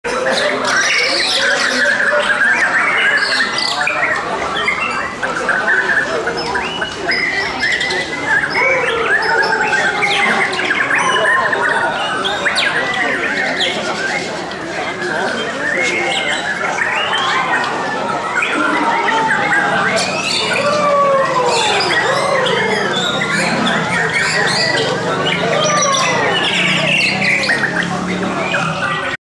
Champion Best of the Best no.7
Pertandingan Seni Suara & Gaya Murai Batu Borneo
Anjuran Persatuan Belia Sepakat Menumbok
Sempena Pesta Nelayan
Pasar Am Mempakul, Menumbok
01st Nov 2014
64 ekor burung yg bertanding